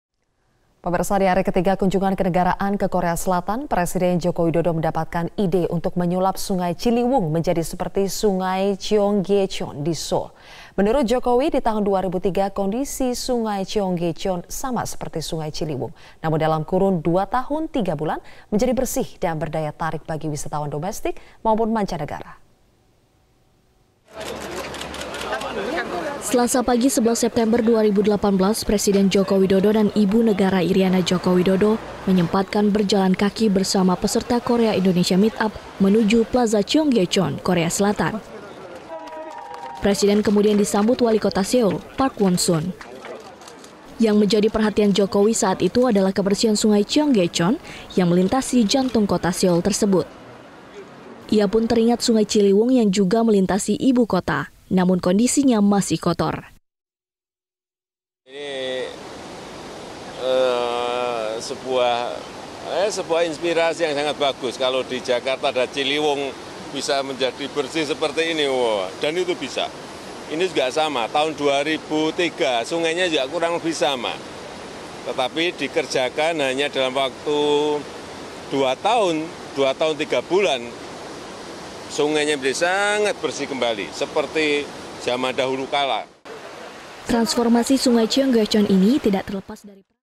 Indonesia Bisa ! Jokowi : Beningnya Sungai Cheonggyecheon Di Seoul Bisa Ditiru Sungai Ciliwung
Dihari ketiga kunjungan kenegaraan presiden Joko Widodo mendapatkan ide untuk menyulap sungai Ciliwung menjadi seperti sungai Cheonggyecheon di Seoul, Korea Selatan. Menurutnya ditahun 2003 sungai Cheonggyecheon sama seperti sungai Ciliwung, namun dalam kurun dua tahun tiga bulan menjadi bersih dan menjadi daya tarik wisatawan domestik maupun mancanegara.